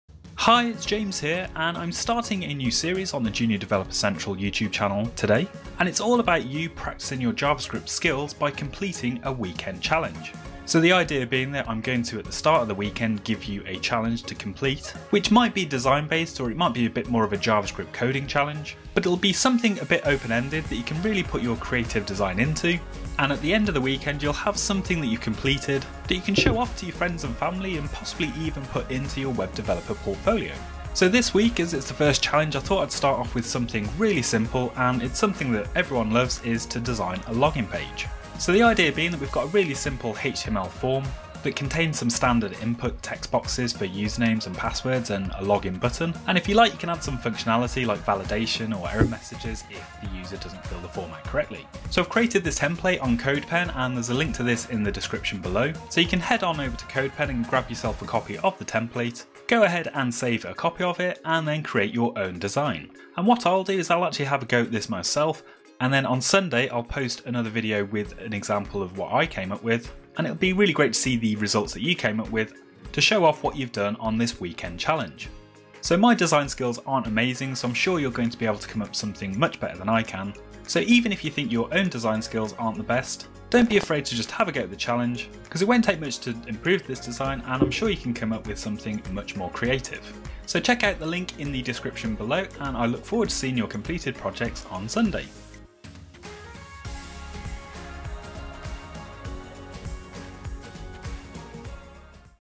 Weekend Coding Challenge - 26/06/2020. - Design A Login Form
This week's challenge is all about designing a login form!

So, this is a start of a new series here on the Junior Developer Central channel where i'll be giving you a web development challenge for you to complete over the weekend.

Then on Sunday, i'll show you what I came up with and give you an opportunity to share details of your work.

This first week is going to be a simple one, to design a login form and you get creative with how you want this to look.

I forgot to say in the video, of course, feel free to use Bootstrap / Material or any other framework you like to create your design 👍

So good luck, and I look forward to seeing your login form designs! Channel Handle @codebubb